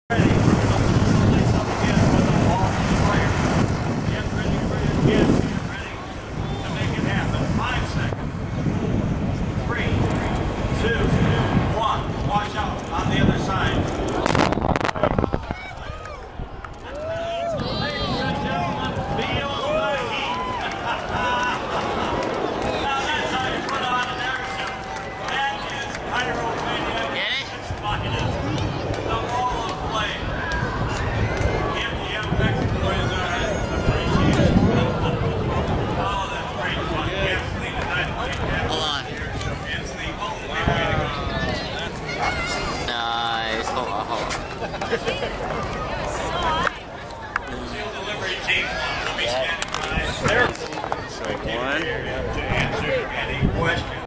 WALL OF FIRE! [Blues on the Bay 2007]
The Wall of Fire during the Sunday show at Kaneohe Bay, HI, in 2007.

~Henry and Kenneth

NOTES: : ] Recommended Settings To YT